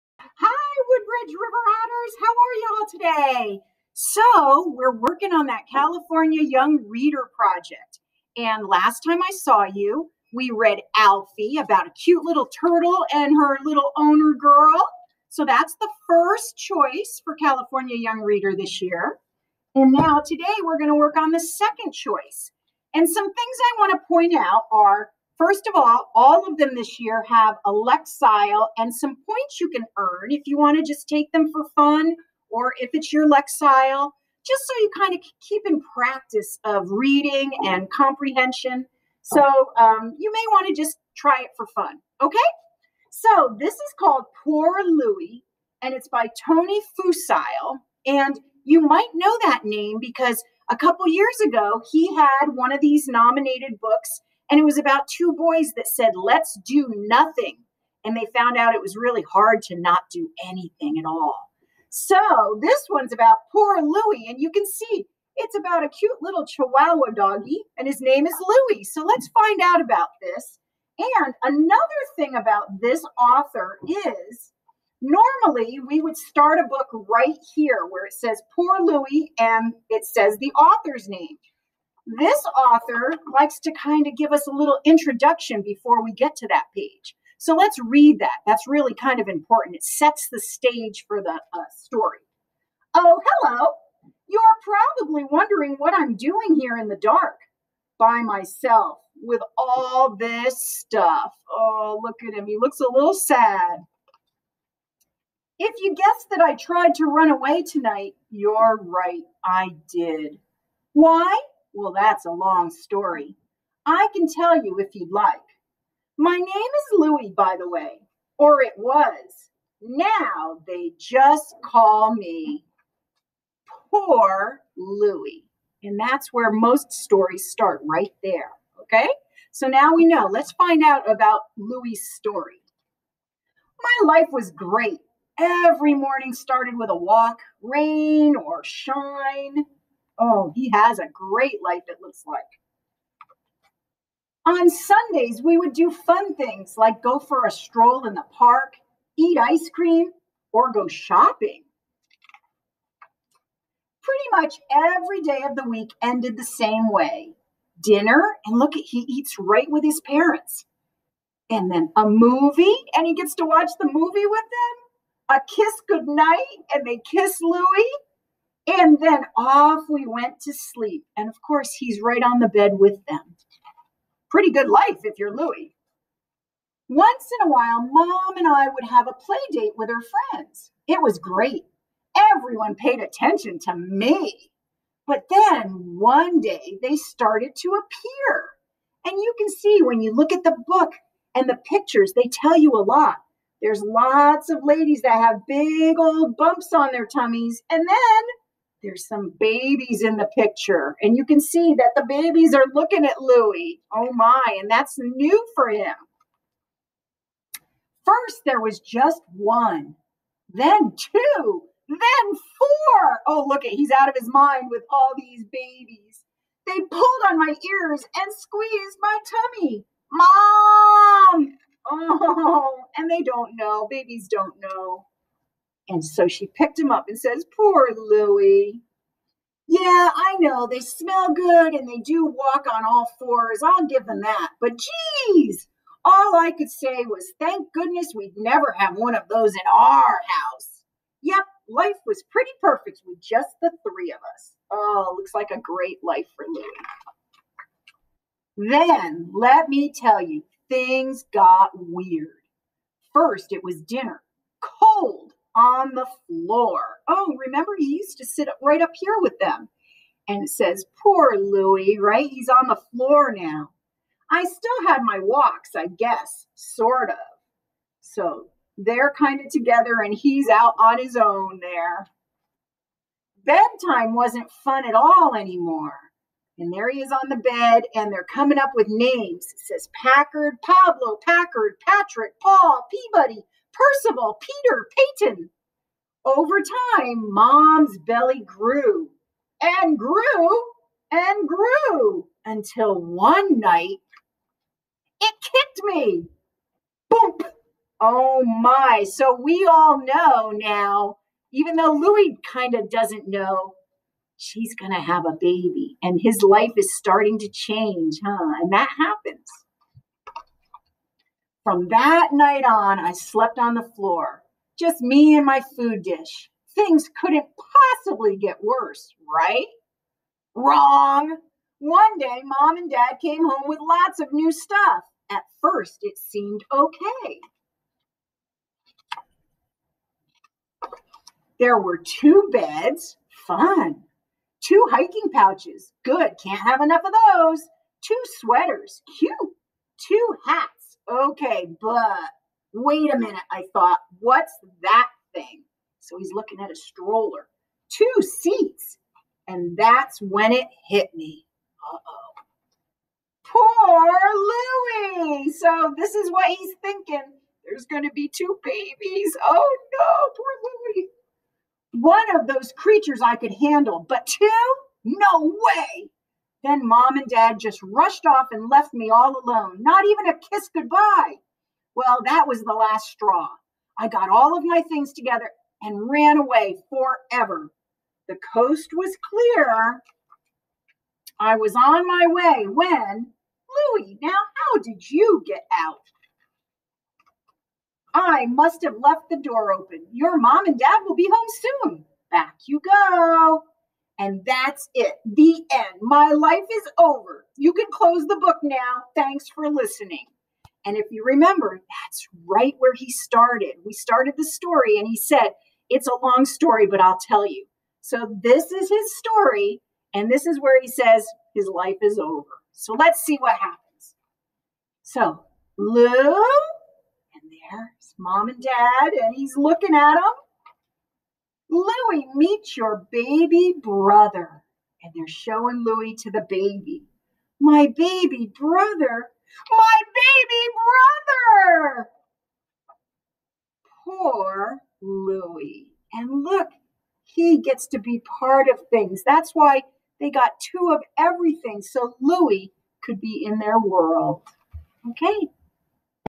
Mrs. Skagg's reads Poor Louie
Fucile, T. (2017). Poor Louie (Illustrated ed.). Candlewick.